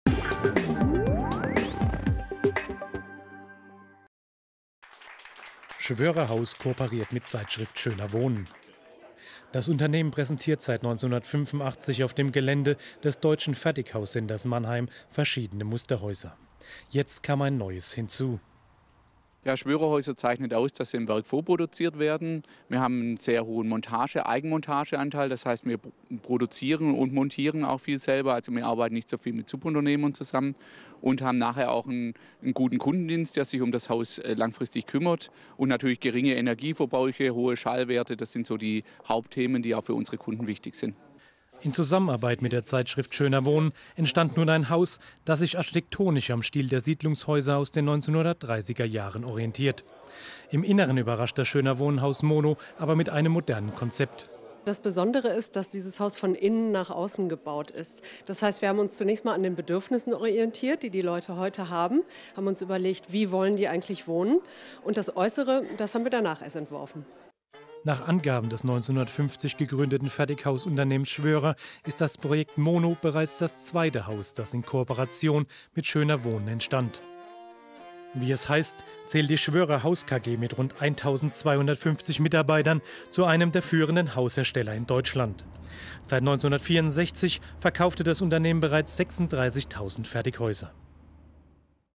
Eröffnung SCHÖNER WOHNEN Haus-MONO
Am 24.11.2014 wurde ein das SCHÖNER WOHNEN-Haus MONO im Fertighauscenter Mannheim eröffnet. SchwörerHaus geht mit der zweiten Kooperation mit SCHÖNER WOHNEN, Europas größter Wohnzeitschrift, neue Wege. Ein schlichtes Haus mit großen inneren Qualitäten zu einem bezahlbaren Preis.